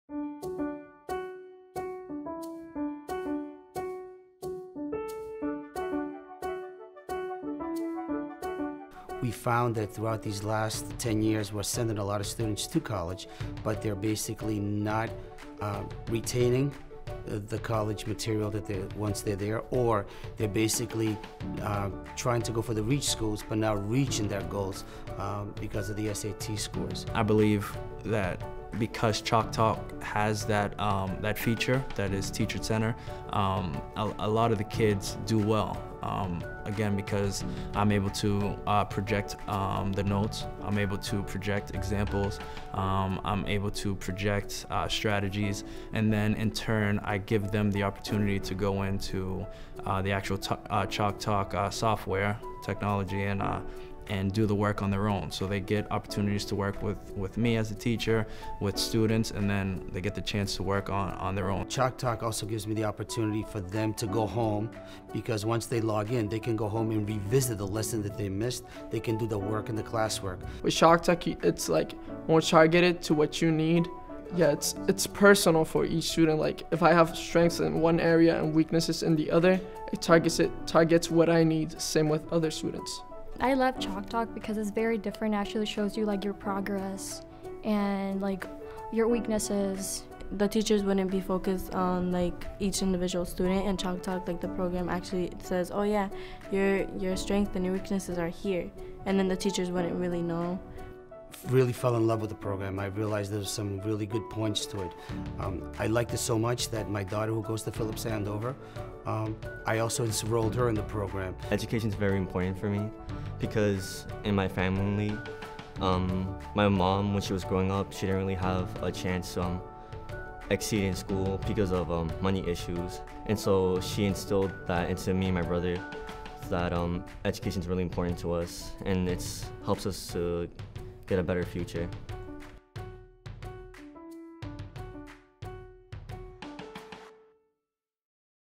ChalkTalk - East Boston High School
At ChalkTalk, we’re helping teachers discover how the most advanced technology in education isn’t one that requires hours of training or upends pedagogy - it’s technology that runs behind the scenes, personalizing instruction while nudging students along a path of continual success.

ChalkTalk is a remarkable new tool that helps every teacher ‘teach amazing.’ Using fully-adaptive technology, it generates a personalized, supplemental curriculum for high school English Language Arts and Math classes that has shown to dramatically improve student learning outcomes, including state test scores.

Using results from a student placement test, ChalkTalk’s machine-learning software generates standards-aligned curriculum customized to fit within the existing class schedule. Lesson plans, instructional slides, walkthrough examples, and small group practice activities are adaptively generated for the teacher, while personalized practice problems and review exercises are adaptively generated for each student. In this way, ChalkTalk acts as the ultimate teacher’s assistant - scaffolding instruction for each student while using class progress to shape future instruction.

Today, ChalkTalk is used by schools across the United States from New York to Hawaii. After just 15 weeks, every school who used ChalkTalk had their students experience 2-6x more growth on state test scores compared to their peers after a full year of traditional instruction.